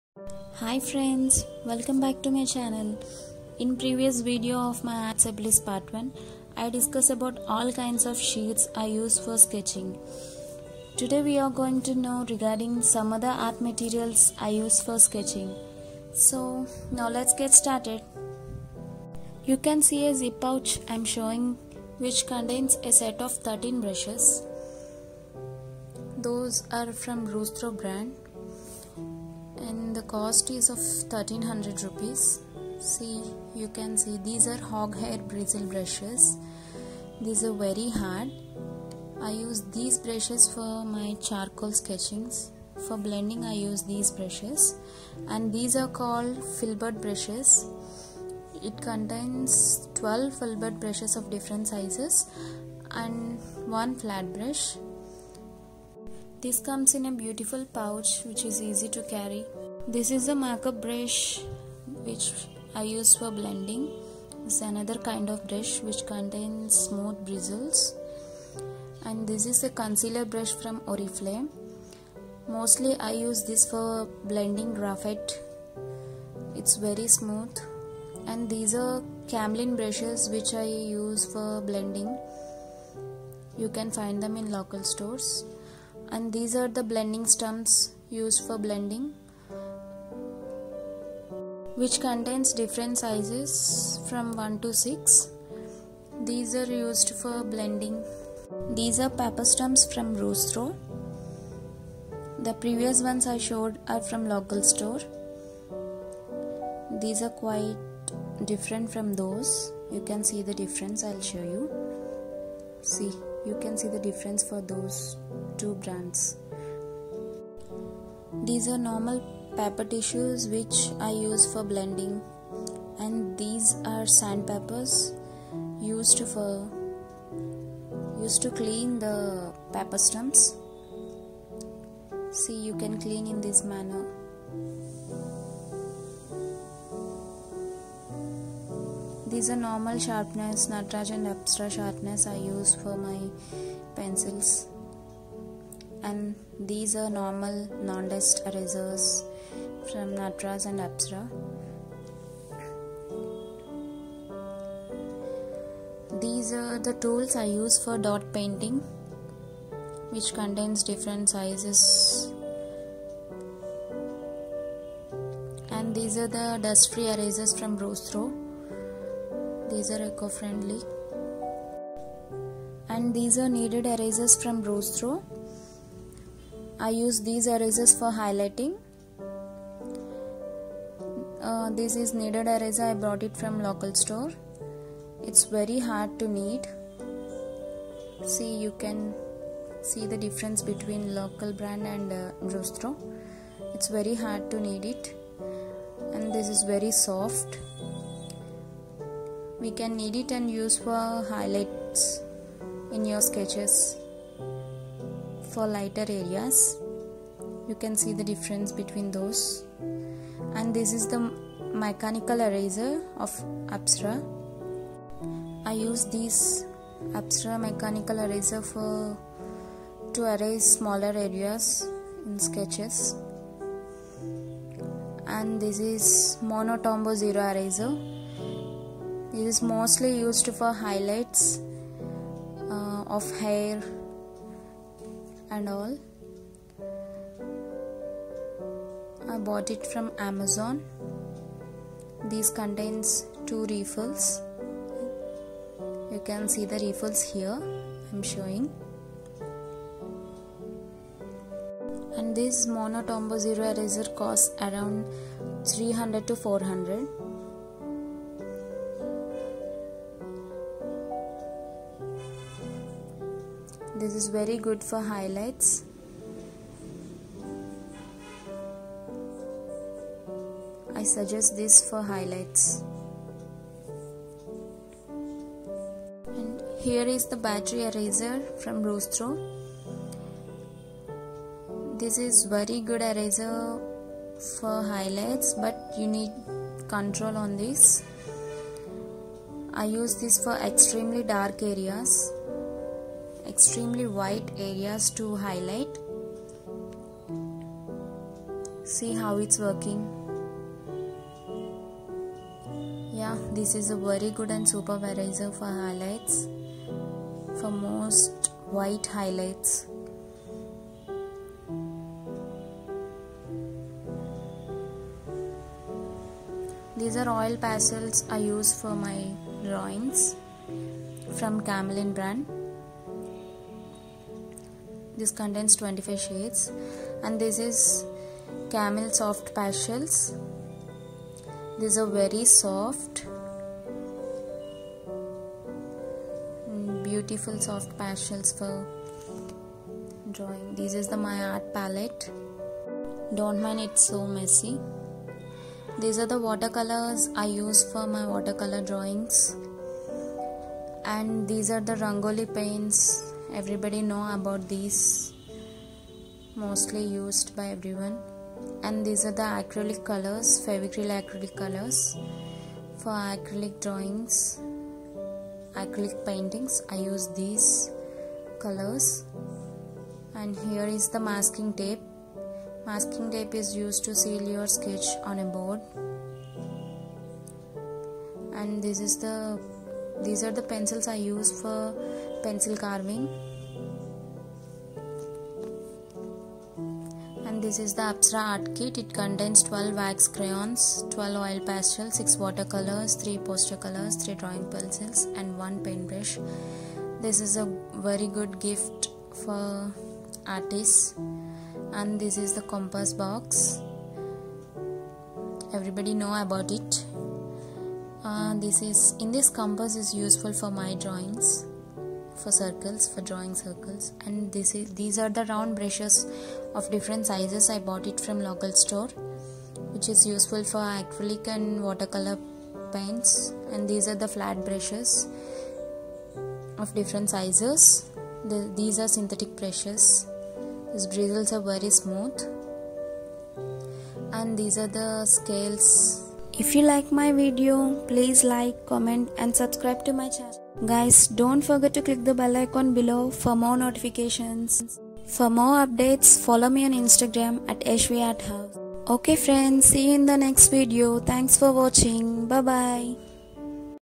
Art supplies collection||part-2||my collection ||Real time video
Hi frnds.
Here is my art supplies collection part 2
If u have any queries regarding the art supplies u can ask me in the comment section below.
If u like my video plss do like and support me.
Yasvi_art_house

Links given down for art supplies mentioned in the video

Brustro hog hair brushes set of 13